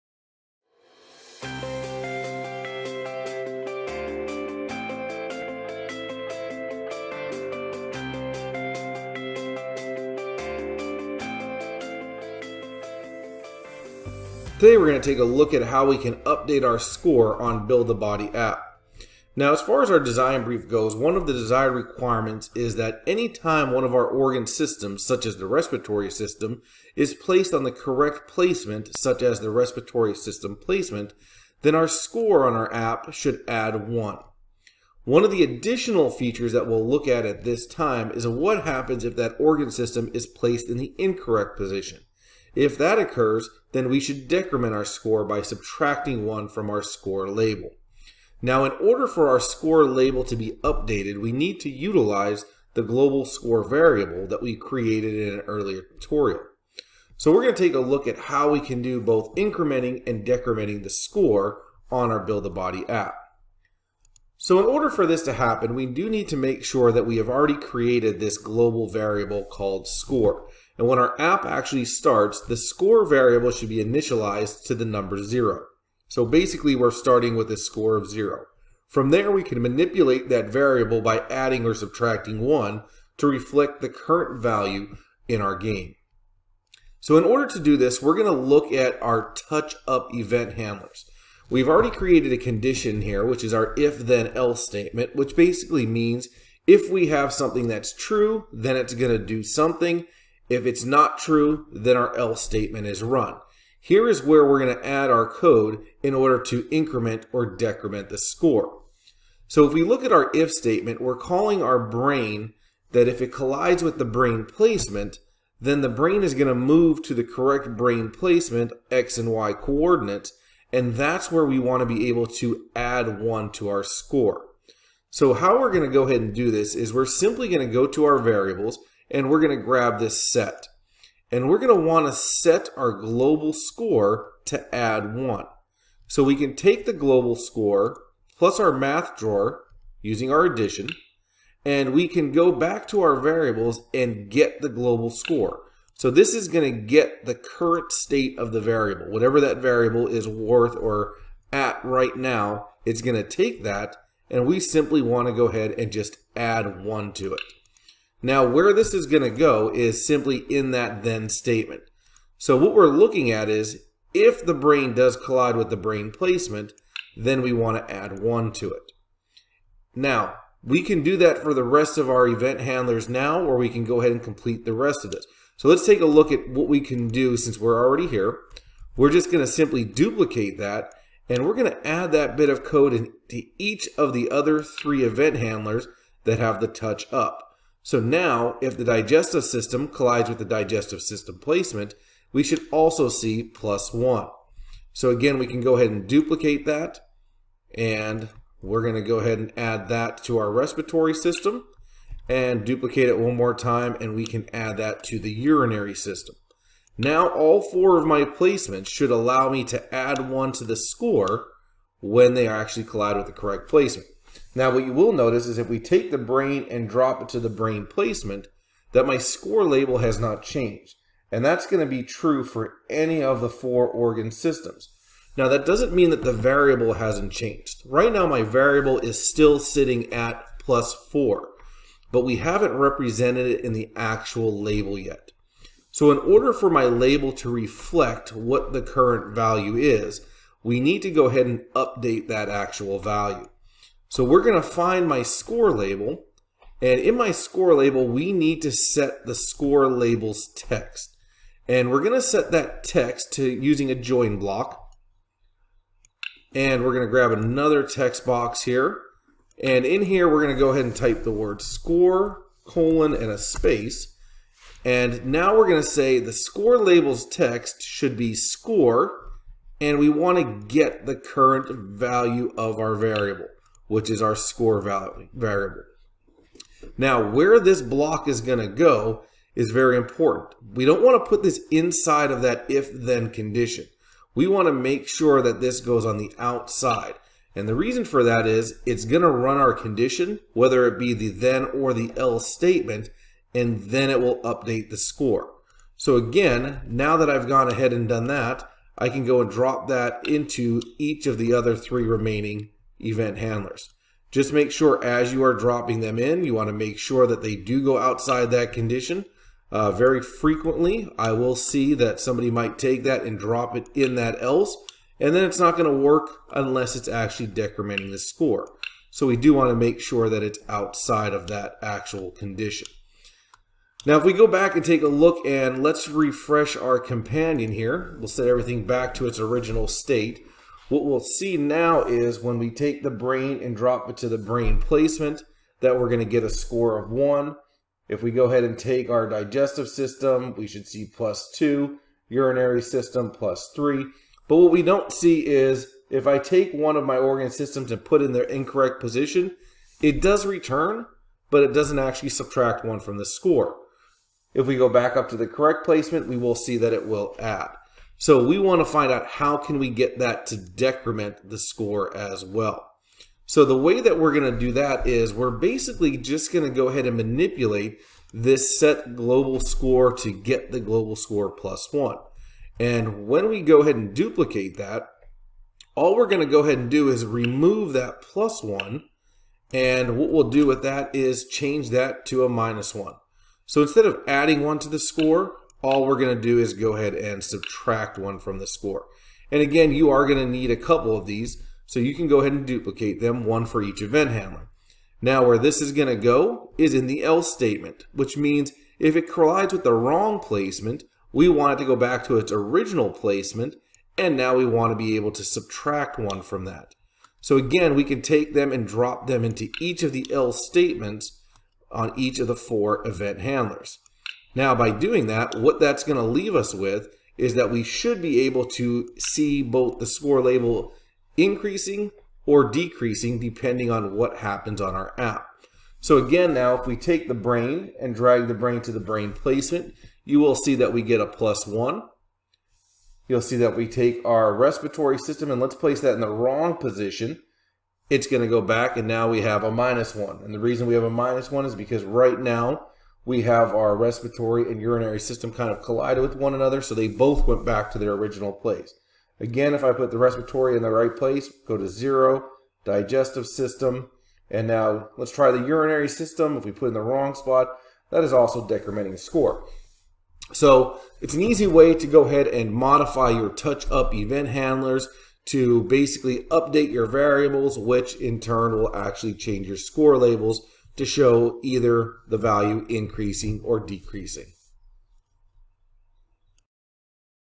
Project 1.8 - Build a Body (Updating the Score)
This video tutorial will demonstrate how to update the Score when the Organ Systems are dragged. The score will either increment or decrement based on their position.